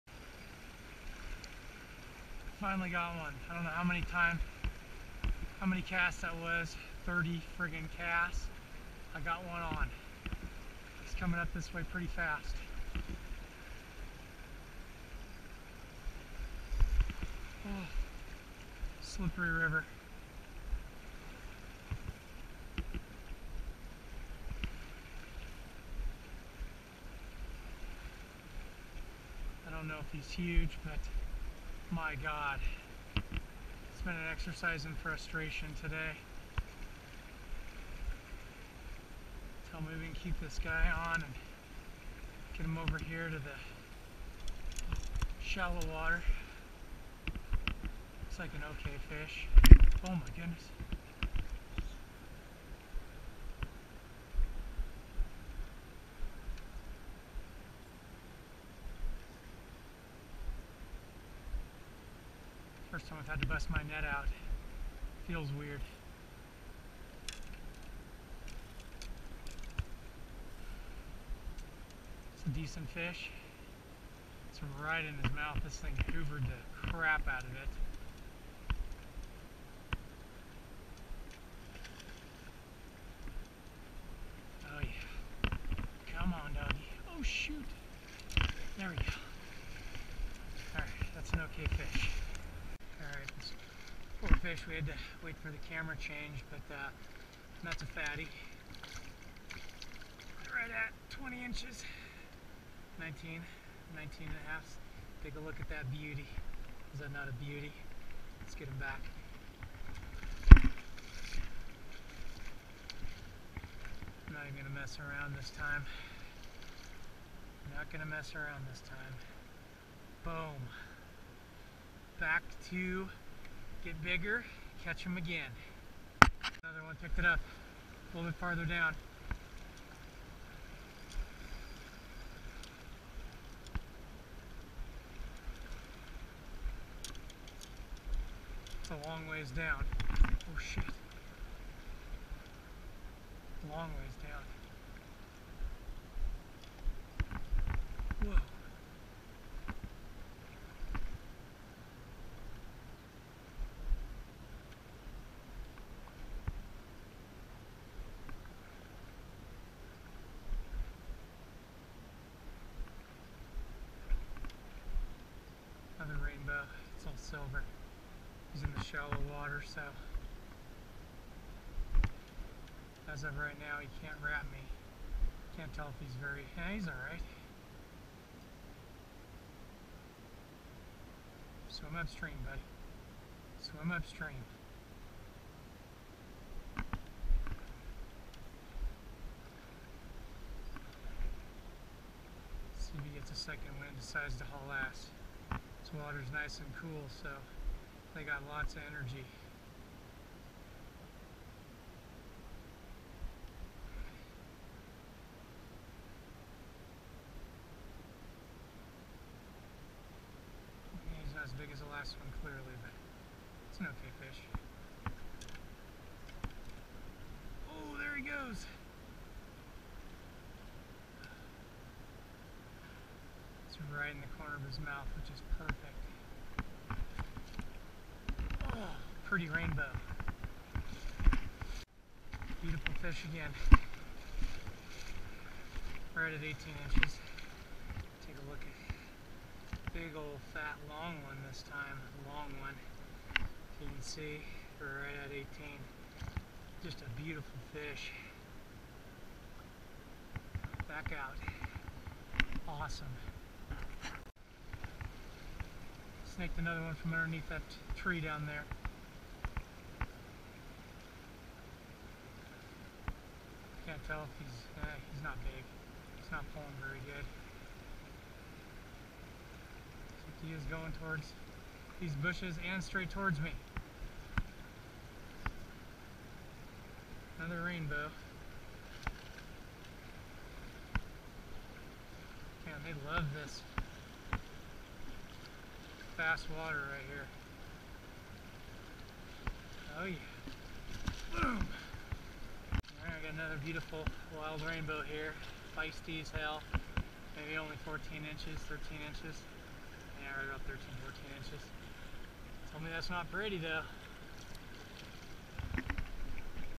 Had to Work for The Truckee River Trout Today - Only Found One Hole Where A Couple Were Hungry
Rough evening.  Walked over a mile stretch of the river trying to find some fish.  Water was low and crystal clear.  Only found one, measly small stretch that was holding any fish that wanted to eat.  Was able to get those into my net!